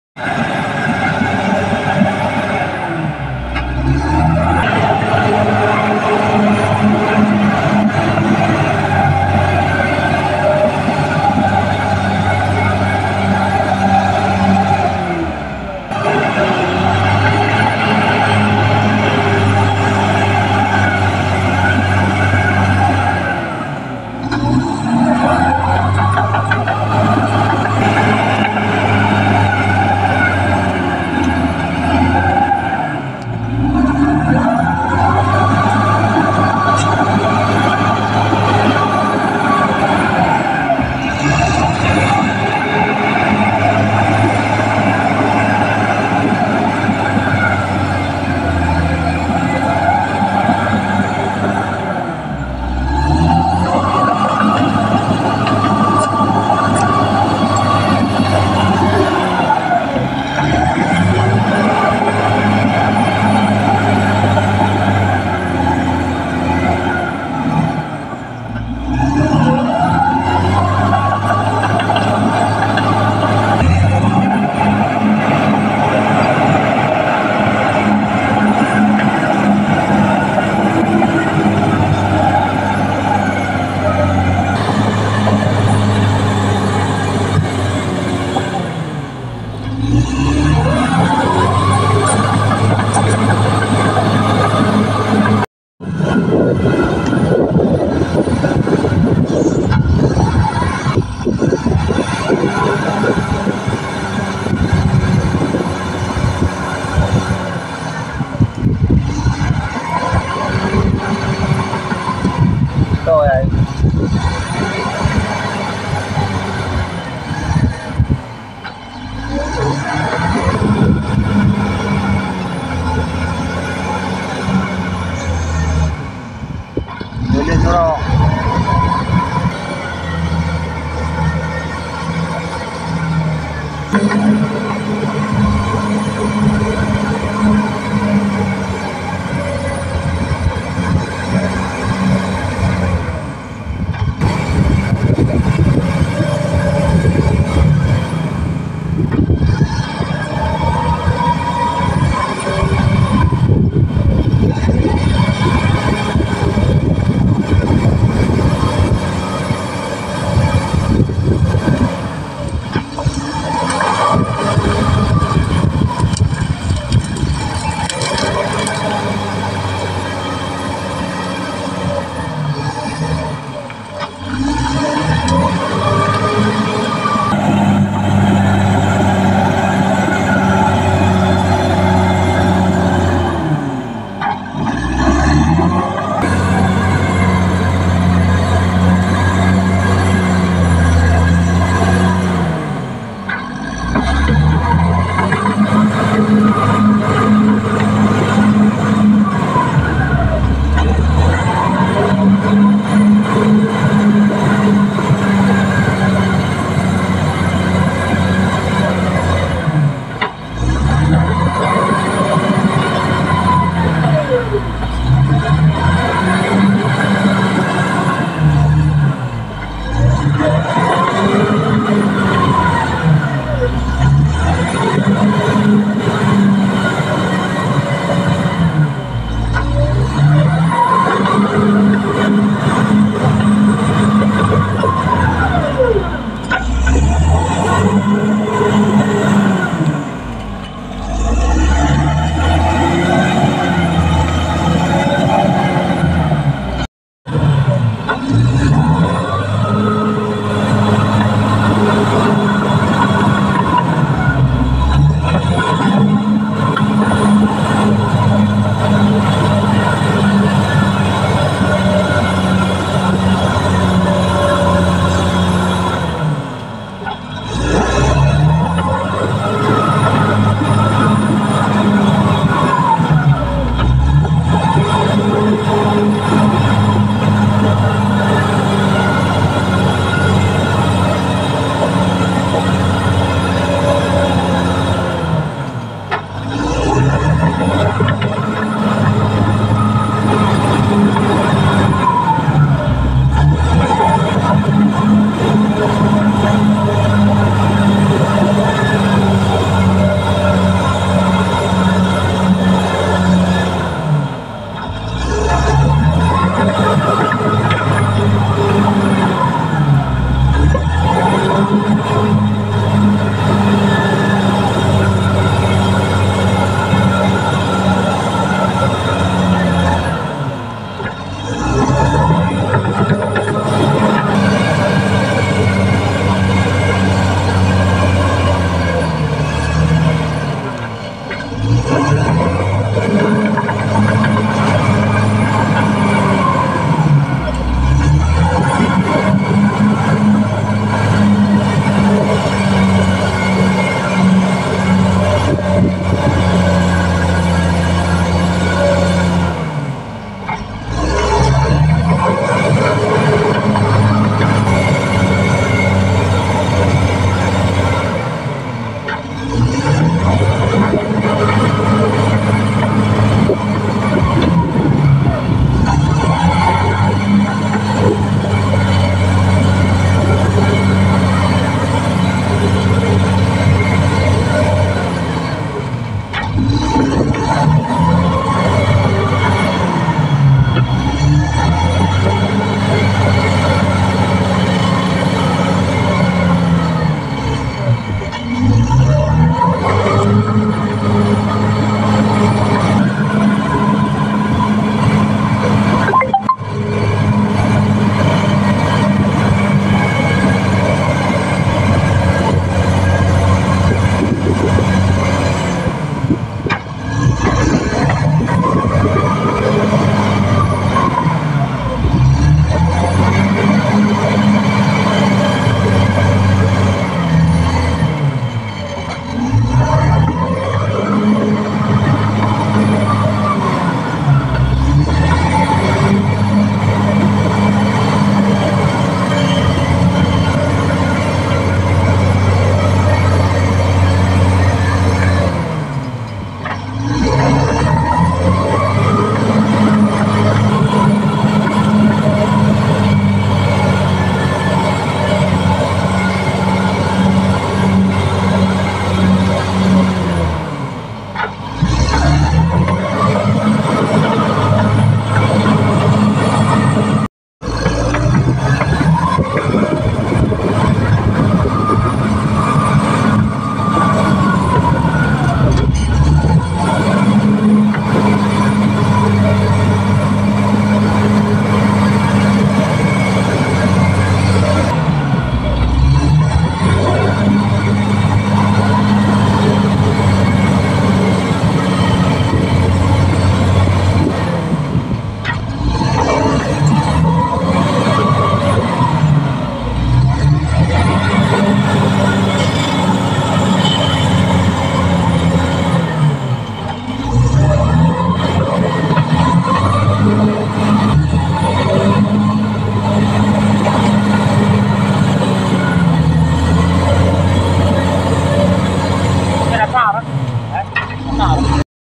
Very extreme paving the way on the edge of the abyss CAT BULLDOZER D6R XL does a very good job
The Caterpillar Bulldozer D6R XL is known for its exceptional performance in extreme conditions and its ability to pave the way effectively. It's a powerful and versatile machine used in various construction and earthmoving applications. For a detailed description and videos showcasing its performance, I recommend searching on YouTube or visiting the official Caterpillar website, where you can find specific information and demonstrations of this bulldozer model.